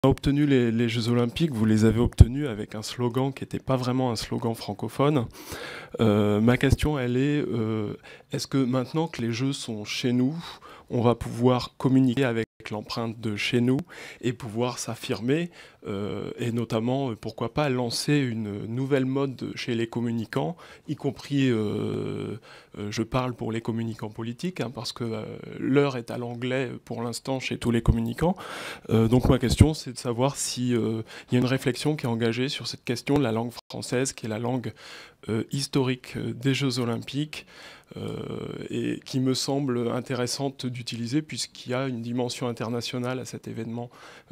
Jeux Olympiques en français... sauf en France ?
Le français est la langue des J.O., sauf pour la France, honteuse un peu de sa langue ?

Mercredi 31 janvier 2018
Question à Tony Estanguet, organisateur des J.O de Paris 2024 : pour obtenir les jeux, de l'anglais, de la com' en anglais, des slogans en anglais, des communicants en anglais... Mais l'histoire des Jeux Olympiques s'est écrite en français ! Avons nous le français honteux en sport ? Le baron de Coubertin est-il honoré d'entendre le slogan des jeux en France "Made for sharing" ?